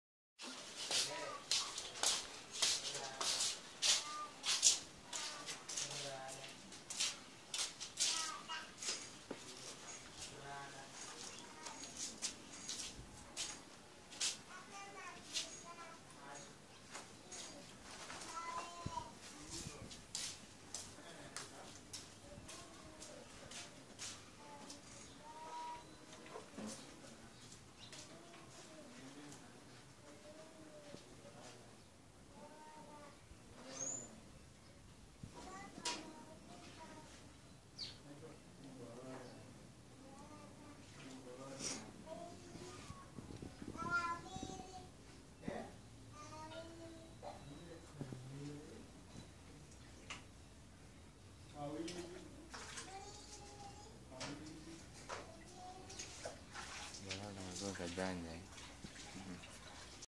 Painting with oil paints. Finish up.
Brief showing how to finalising on your portrait using  oil paints